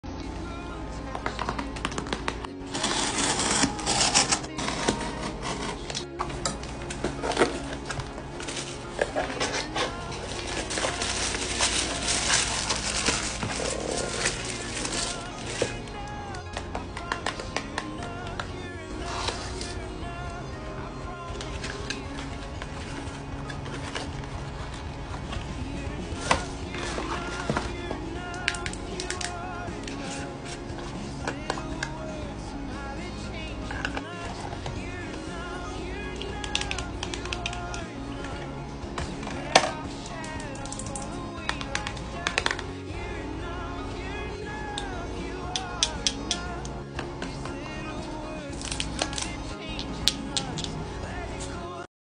HUDA BEAUTY, KAYALI & WISHFUL SKIN ASMR UNBOXING
Hey lovelies! Peep my latest @Hudabeauty haul! I also picked up Wishful’s newest launches as well as the most recent fragrances from Kayali to complete my collection! Hope you all are having a great holiday and enjoy this ASMR moment💕